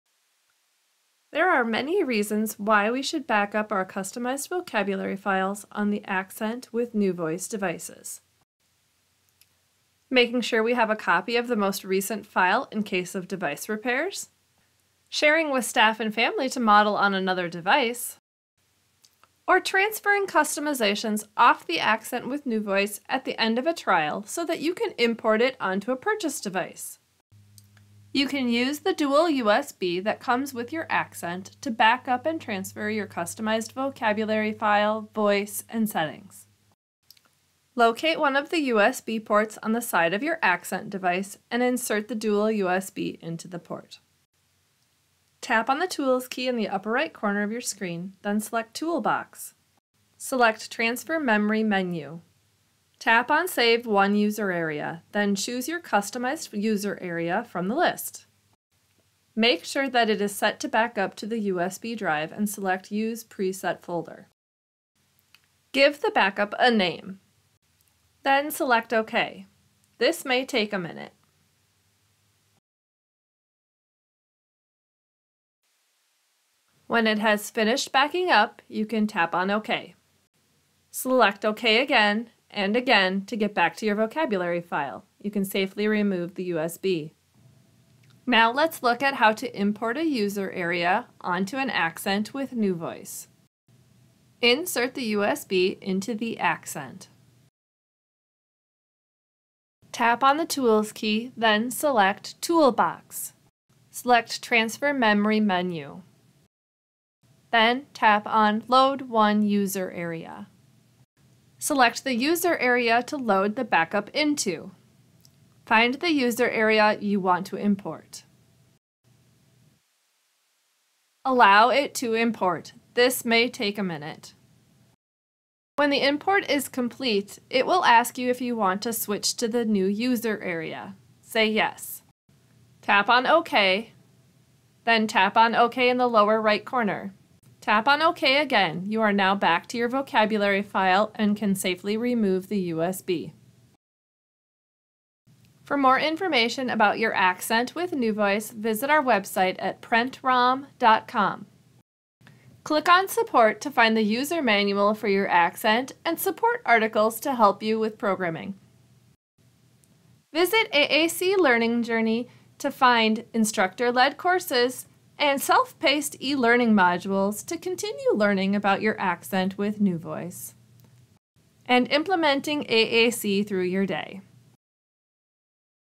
Backing Up Accent with NuVoice with a USB
This video shows how to backup a user on an Accent with NuVoice using a USB drive, and how to load that user back onto an Accent device.